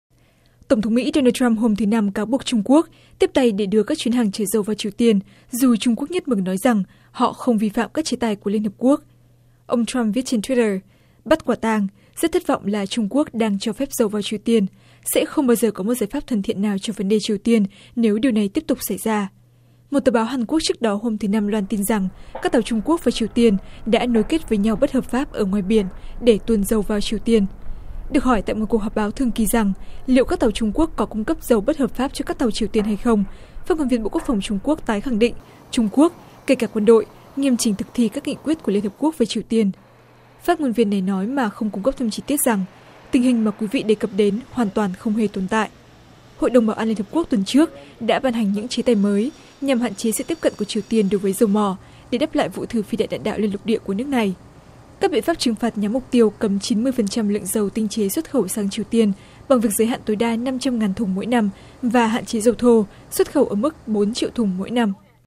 Trump cáo buộc Trung Quốc cho phép tuồn dầu vào Triều Tiên
Tin tức Nếu không vào được VOA, xin các bạn hãy vào để vượt tường lửa. Tổng thống Mỹ Donald Trump hôm thứ Năm cáo buộc Trung Quốc tiếp tay để đưa các chuyến hàng chở dầu vào Triều Tiên, dù Trung Quốc nhất mực nói rằng họ không vi phạm các chế tài của LHQ.

Ông Trump viết trên Twitter: "Bắt QUẢ TANG - rất thất vọng là Trung Quốc đang cho phép dầu vào Triều Tiên. Sẽ không bao giờ có một giải pháp thân thiện nào cho vấn đề Triều Tiên nếu điều này tiếp tục xảy ra!"

Một tờ báo Hàn Quốc trước đó hôm thứ Năm loan tin rằng các tàu Trung Quốc và Triều Tiên đã nối kết với nhau bất hợp pháp ở ngoài biển để tuồn dầu vào Triều Tiên.

Được hỏi tại một cuộc họp báo thường kỳ rằng liệu các tàu Trung Quốc có cung cấp dầu bất hợp pháp cho các tàu Triều Tiên hay không, phát ngôn viên Bộ Quốc phòng Trung Quốc tái khẳng định Trung Quốc, kể cả quân đội, nghiêm chỉnh thực thi các nghị quyết của LHQ về Triều Tiên.

"Tình hình mà quý vị đề cập đến hoàn toàn không hề tồn tại," phát ngôn viên này nói mà không cung cấp thêm chi tiết.

Hội đồng Bảo an Liên Hiệp Quốc tuần trước đã ban hành những chế tài mới nhằm hạn chế sự tiếp cận của Triều Tiên đối với dầu mỏ để đáp lại vụ thử phi đạn đạn đạo liên lục địa của nước này.

Các biện pháp trừng phạt nhắm mục tiêu cấm 90 phần trăm lượng dầu tinh chế xuất khẩu sang Triều Tiên bằng việc giới hạn tối đa 500.000 thùng mỗi năm và hạn chế dầu thô xuất khẩu ở mức 4 triệu thùng mỗi năm.

U.S. President Donald Trump accused China Thursday of facilitating oil shipments into North Korea, despite China's insistence it has not violated United Nations sanctions limiting oil shipments to the rogue nation.